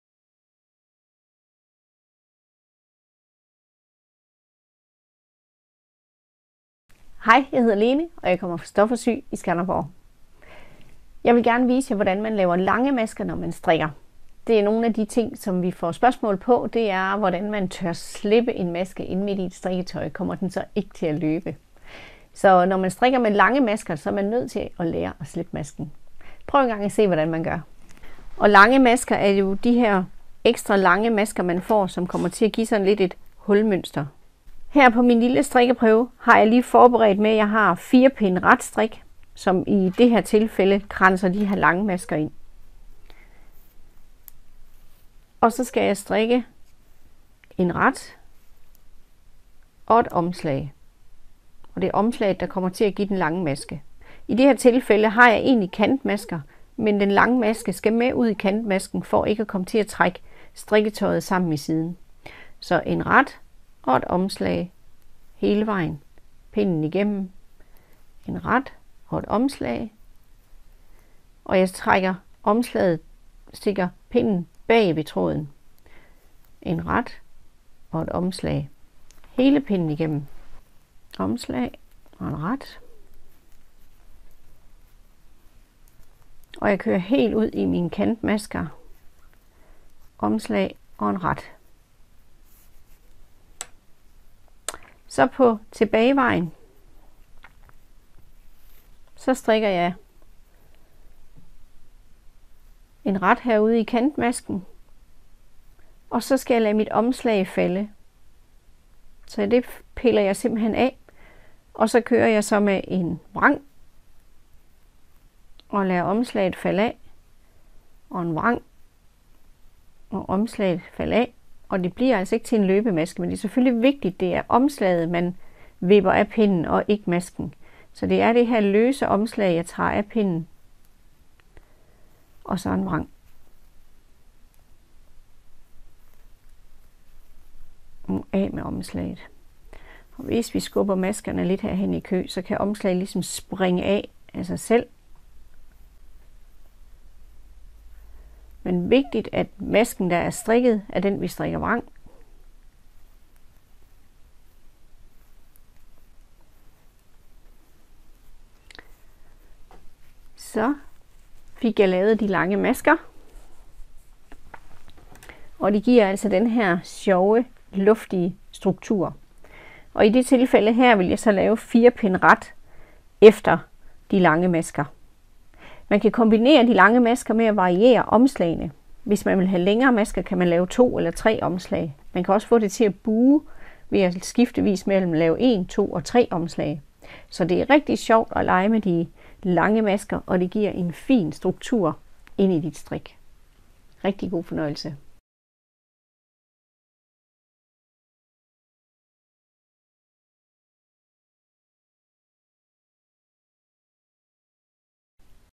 Strik lange masker
At lade en maske falde i et strikketøj, kan være noget angstprovokerende, men ikke hvis det bliver gjort rigtigt, så bliver det en flot effekt.
Lene viser her, hvordan du laver lange masker. Tørklædet hun viser, er fra Lana Grossa Hand-dyed 2